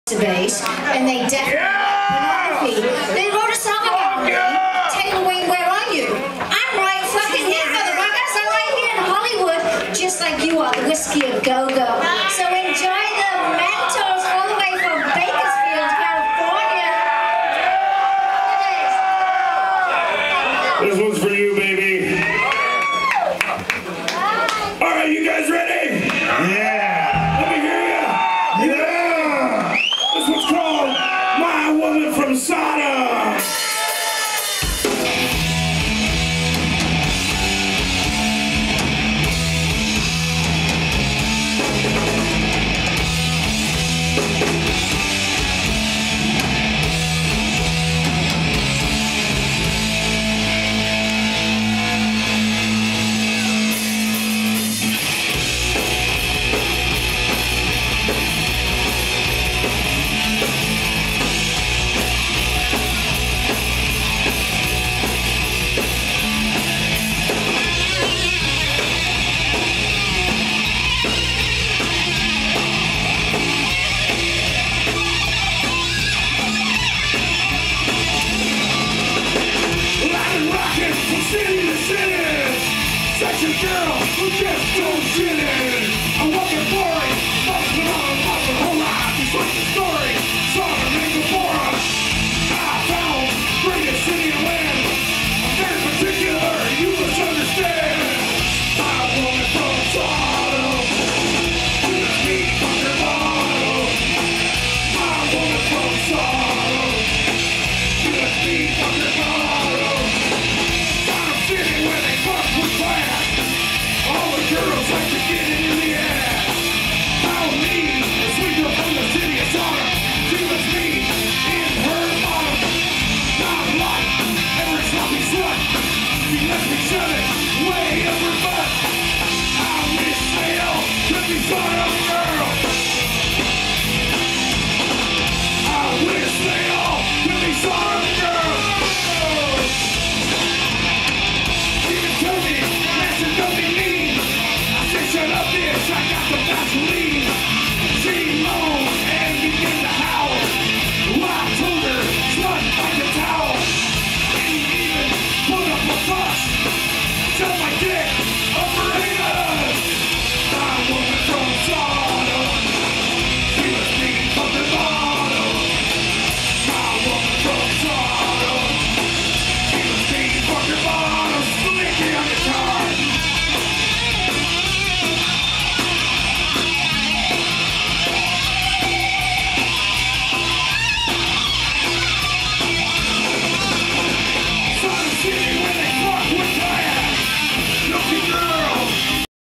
Dr. Heathen Scum's Mentors Being Introduced on Stage by Taylor Wane
Dr. Heathen Scum's Mentors Featuring Marc DeLeon,  being introduced on stage at the Whisky A Go Go in Hollywood, California in Dec 2011 by Adult Movie Superstar Taylor Wayne.